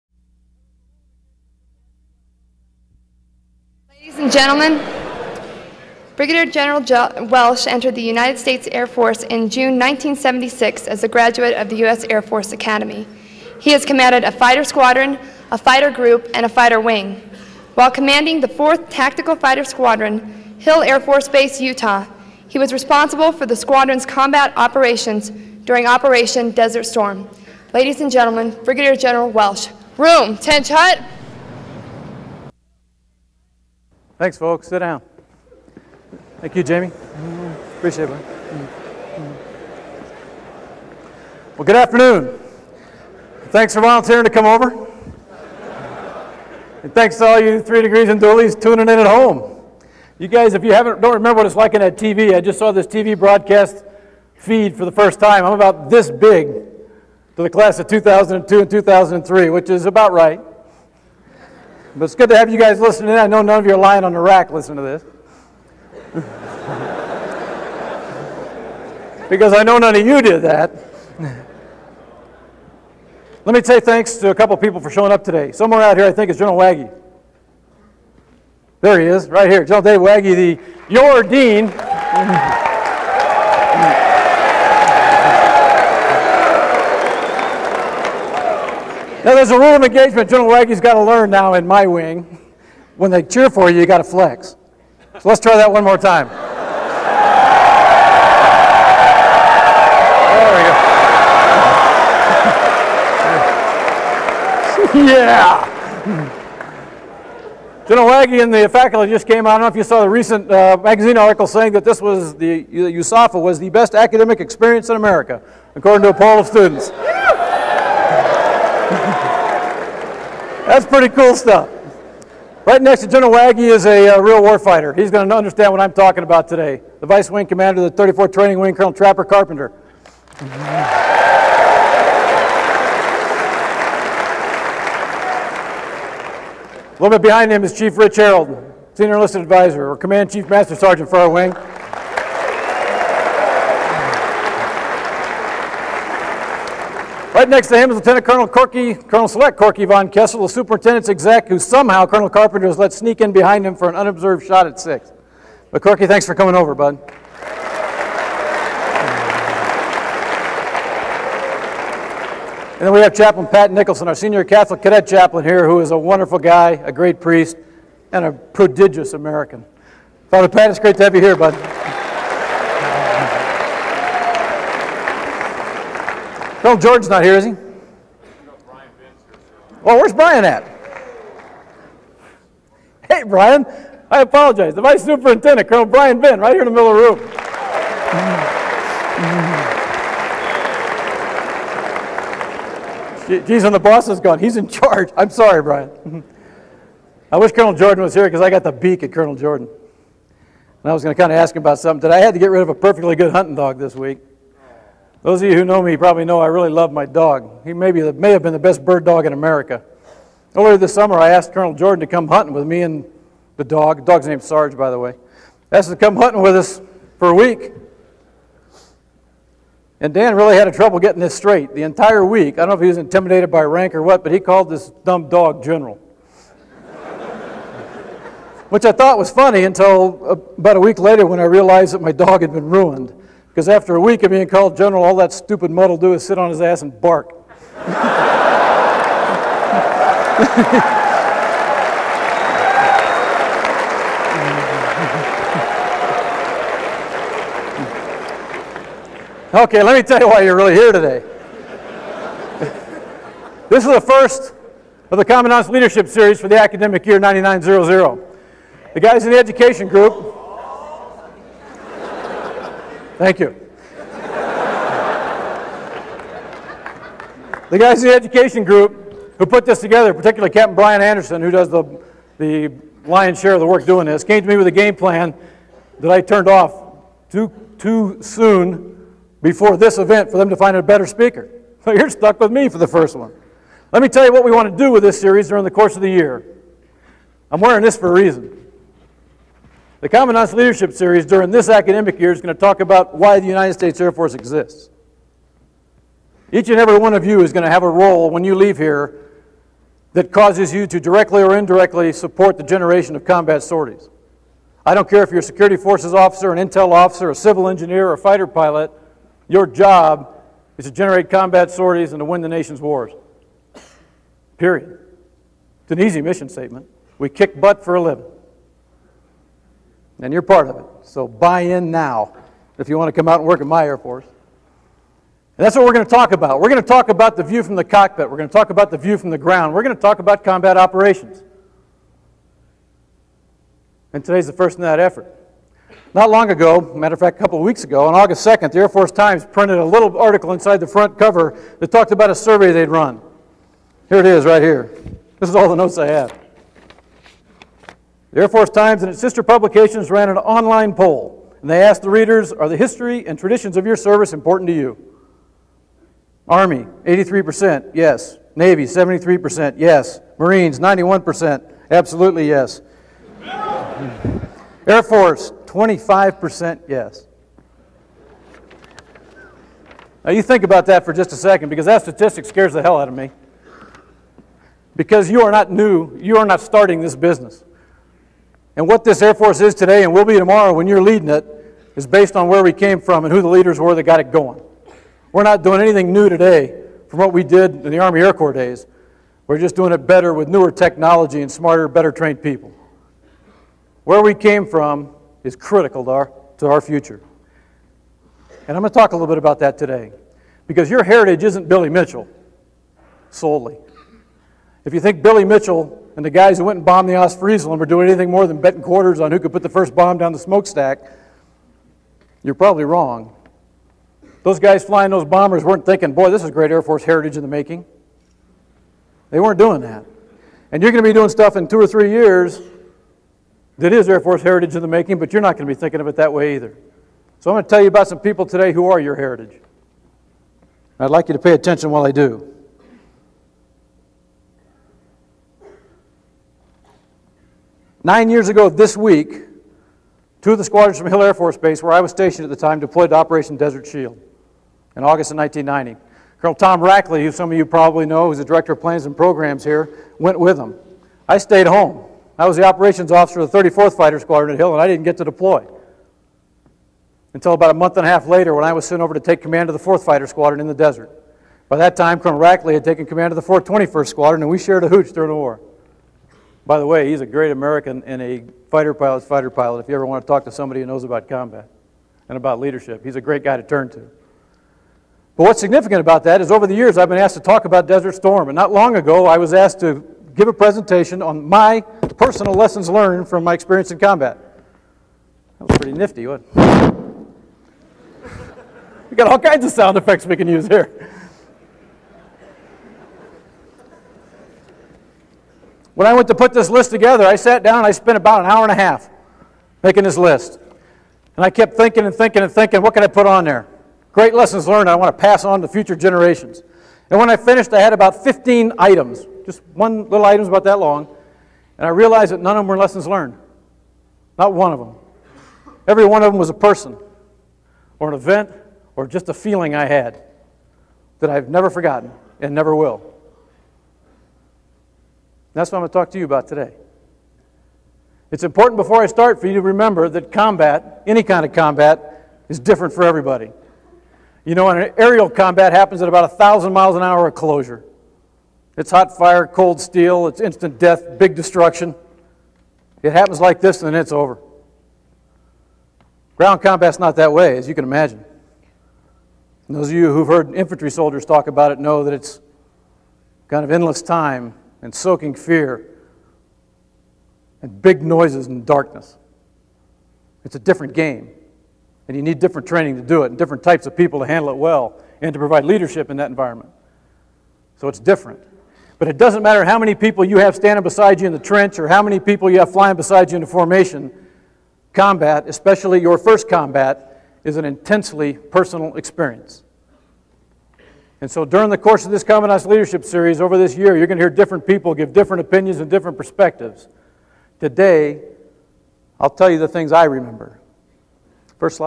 Brig Gen Mark Welsh Speech to USAFA, Part 1, 4:3
Speech to cadets on 8-26-99